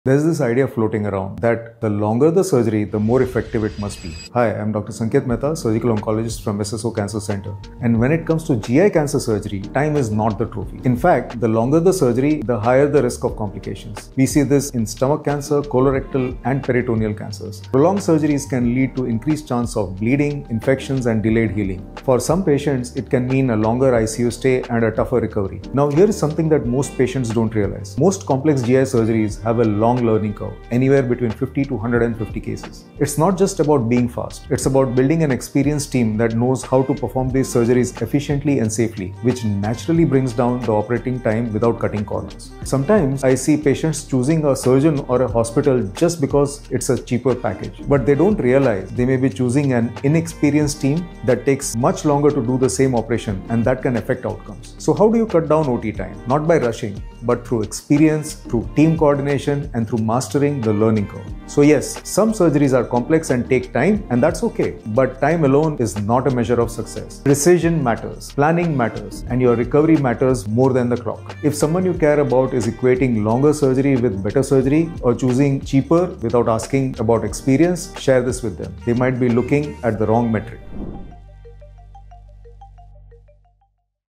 Longer Surgery ≠ Better Surgery
Longer ≠ better — especially in GI cancer surgery:
🕐 Long surgeries = higher risk (bleeding, infections, delayed recovery)
 📈 Learning curve matters — 50–150 cases to master complex GI surgeries
 ✅ Experienced teams = safer + more efficient surgery
 ⚠️ Choosing cheap packages may mean inexperienced teams and longer, riskier surgeries
 🎯 Outcome depends on precision — not just hours in OT
 ✅ Good surgery = right plan, right team, right execution
🎥 Dr. Sanket Mehta explains why surgical success ≠ time spent in OT
 📩 Share this with someone who needs to hear this truth